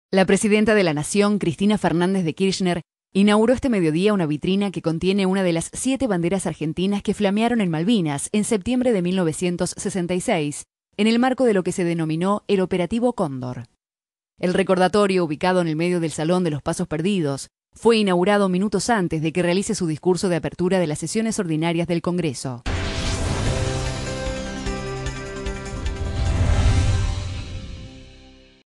La Presidenta inauguro vitrina con bandera del Operativo Condor
La presidenta de la Nación, Cristina Fernández de Kirchner inauguró este mediodía una vitrina que contiene una de las siete banderas argentinas que flamearon en Malvinas en septiembre de 1966, en el marco de lo que se denominó el Operativo Cóndor.
    El recordatorio, ubicado en el medio del Salón de los Pasos
Perdidos, de espaldas a la puerta que comunica con la Cámara de
Senadores, fue inaugurado minutos antes de que realice su discurso de apertura de las sesiones ordinarias del Congreso